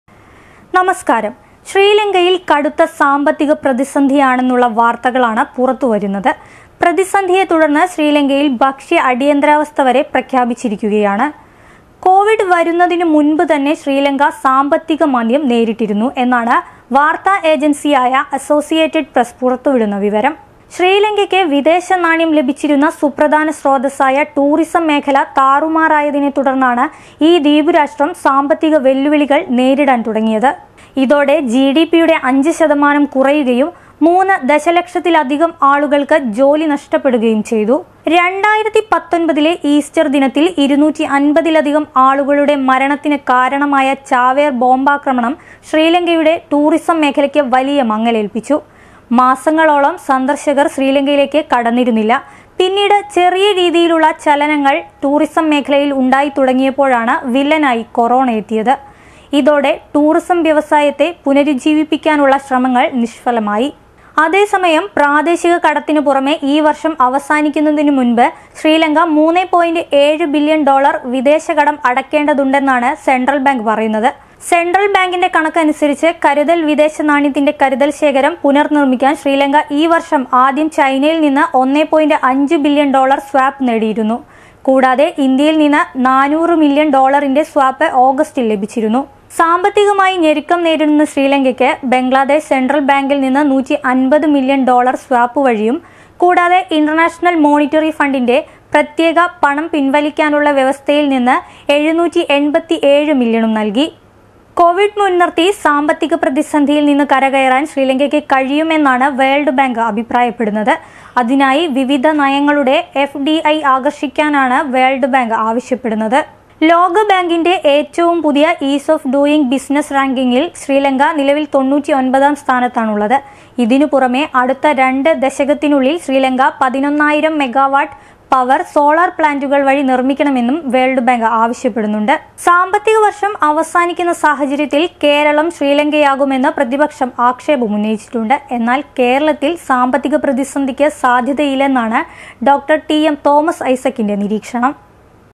സാമ്പത്തിക ഞരുക്കത്തിൽ 'ശ്രീ' മാഞ്ഞ് ലങ്ക. SRILANKA
2019ലെ ഈസ്റ്റര്‍ ദിനത്തില്‍, 250 ലധികം ആളുകളുടെ മരണത്തിന് കാരണമായ ചാവേര്‍ ബോംബാക്രമണം ശ്രീലങ്കയുടെ ടൂറിസം മേഖലക്ക് വലിയ മങ്ങലേൽപ്പിച്ചു. മാസങ്ങളോളം സന്ദര്‍ശകര്‍ ശ്രീലങ്കയിലേക്ക് കടന്നിരുന്നില്ല. പിന്നീട് ചെറിയ രീതിയിലുള്ള ചലനങ്ങള്‍ ടൂറിസം മേഖലയിൽ ഉണ്ടായി തുടങ്ങിയപ്പോഴാണ് വില്ലനായി കൊറോണ എത്തിയത്.